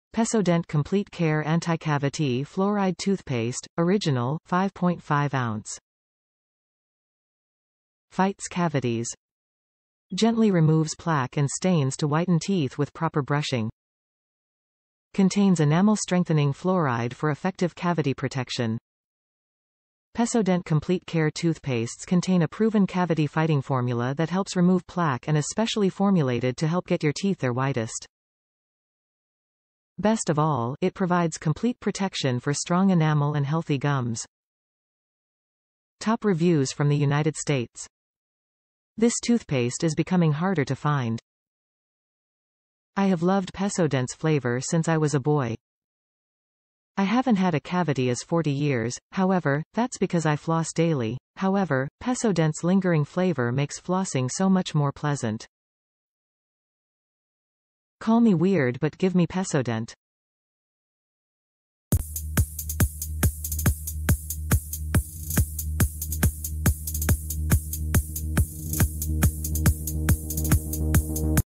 Pepsodent Complete Care Anticavity Fluoride Toothpaste, Original, 5.5 Ounce (Pack of 6)
Pepsodent Complete Care Anticavity Fluoride Toothpaste, Original, 5.5 Ounce
Fights cavities.
Gently removes plaque and stains to whiten teeth with proper brushing.
Contains enamel-strengthening fluoride for effective cavity protection.
Pepsodent Complete Care toothpastes contain a proven cavity fighting formula that helps remove plaque and is specially formulated to help get your teeth their whitest. Best of all, it provides complete protection for strong enamel and healthy gums.
This toothpaste is becoming harder to find.
I have loved Pepsodent's flavor since I was a boy. I haven't had a cavity is 40 years; however, that's because I floss daily; however, Pepsodent's lingering flavor makes flossing so much more pleasant. Call me weird but give me Pepsodent!!!